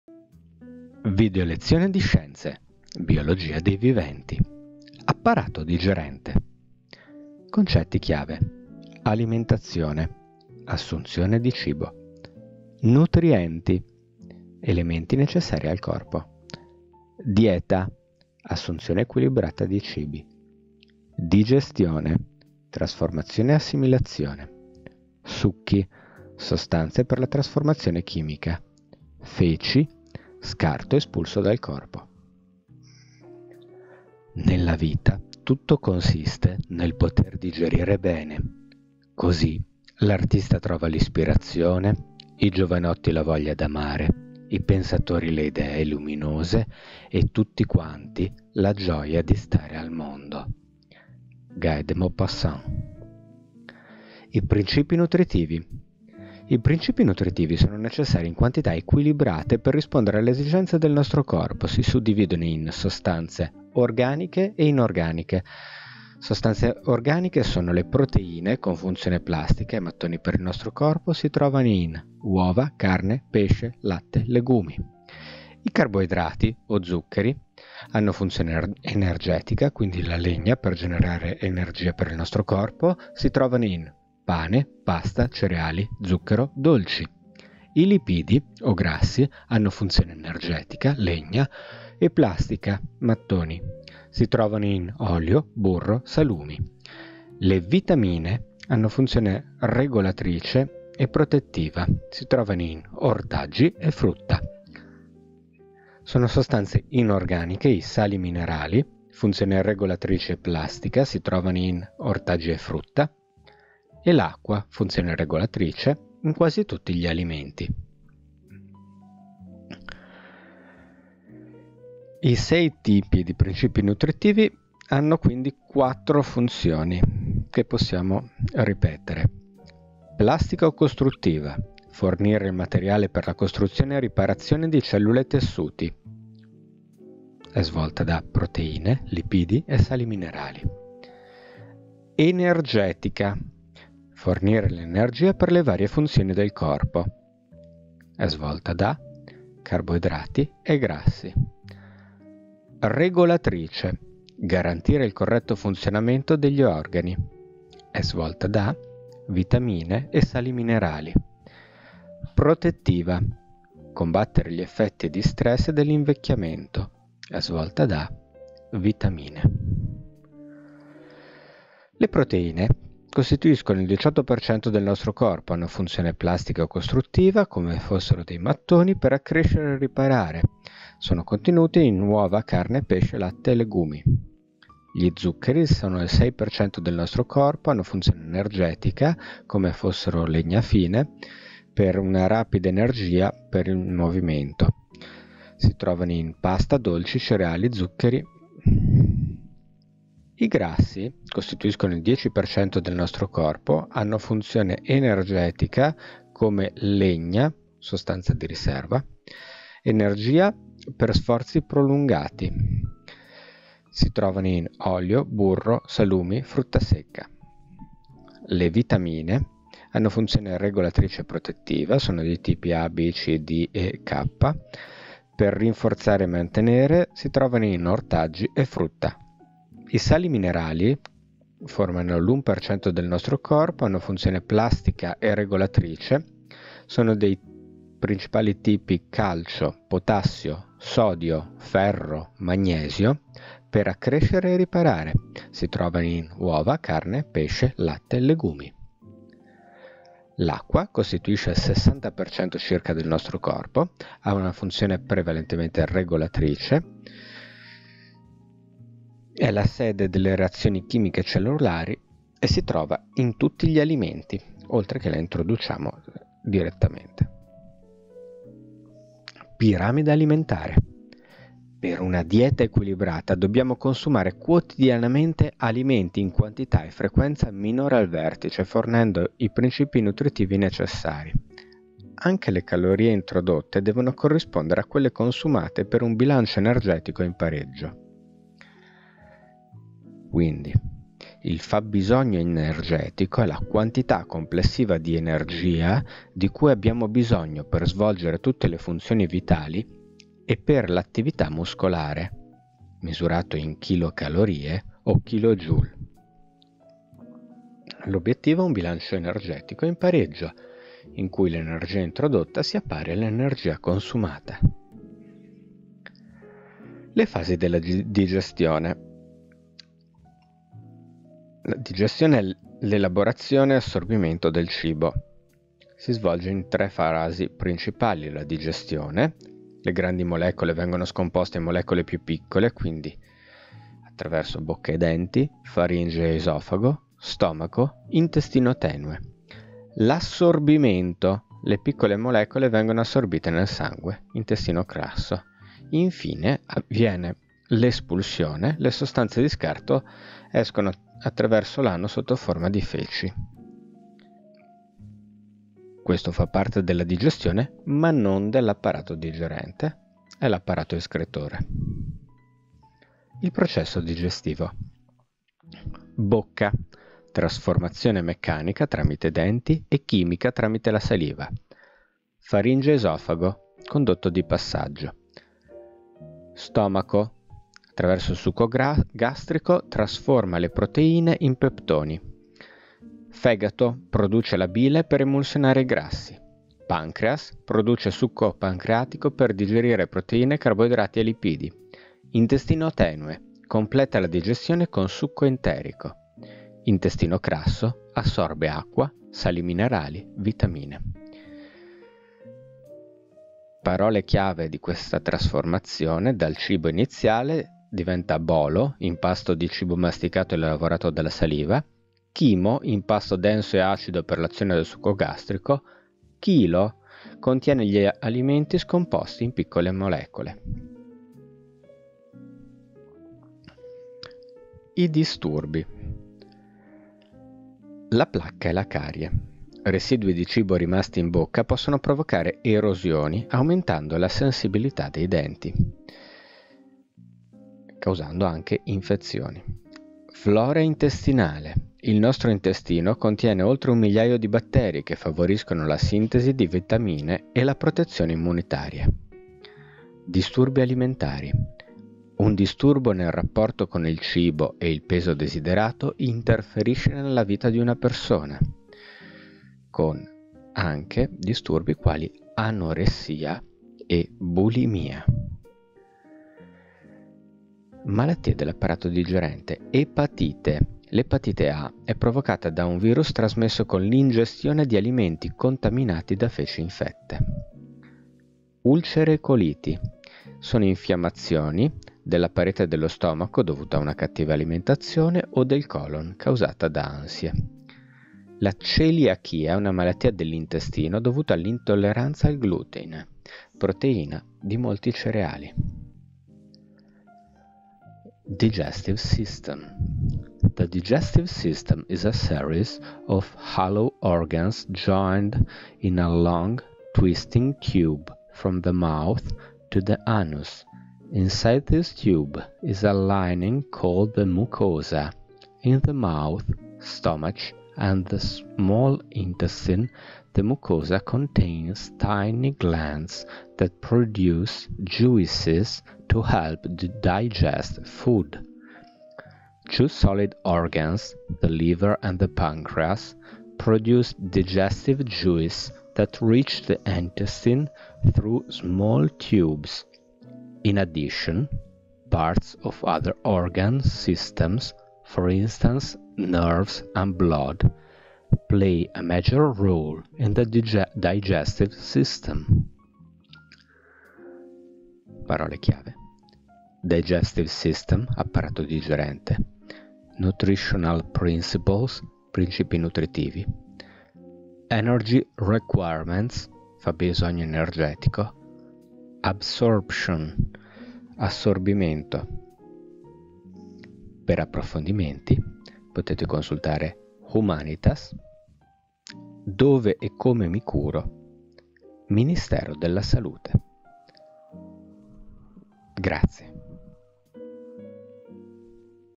SCIENZE - Apparato Digerente
Videolezione di sintesi e consolidamento delle conoscenze sull'argomento "Corpo umano - Apparato Digerente":
- principi nutritivi
- piramide alimentare
- fabbisogno energetico
- digestione
- disturbi
- malattie.